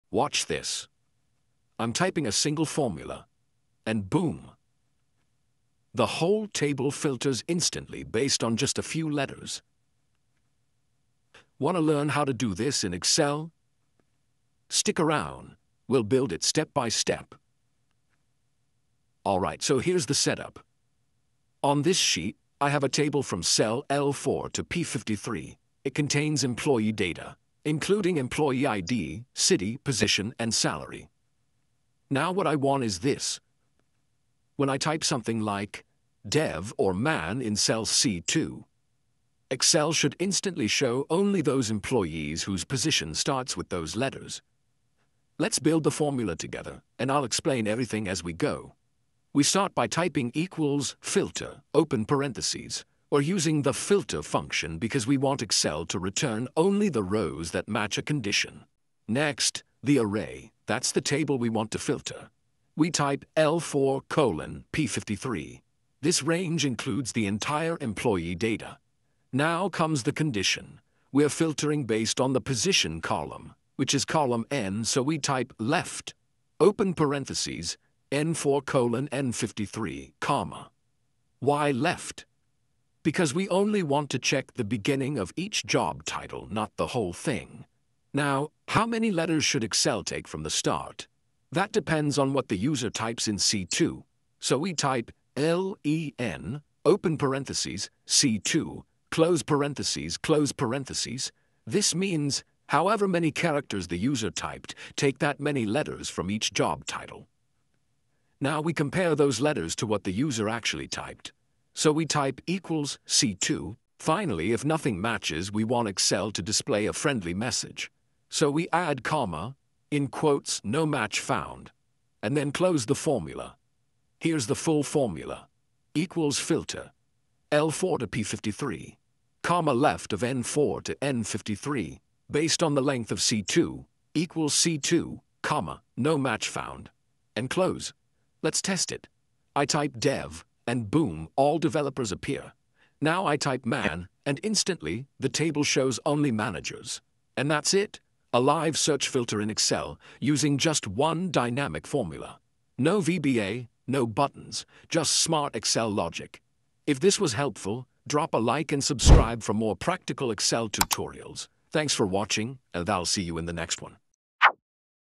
Live Search Filter in Excel with FILTER + LEFT | Dynamic Formula (No VBA!)
Want to create a dynamic search box in Excel without using VBA or macros? In this tutorial, I’ll show you how to build a live search filter using just one formula — combining the powerful FILTER and LEFT functions.

You'll learn:
✅ How to build a dynamic Excel search bar
✅ How to filter a table as you type
✅ The logic behind FILTER + LEFT + LEN
✅ How to search by job position or any column
✅ No VBA, no buttons — just pure Excel magic!

📌 Formula Used:
=FILTER(L4:P53, LEFT(N4:N53, LEN(C2)) = C2, "No match found")

Perfect for:

Dashboards

Reports

Interactive Excel projects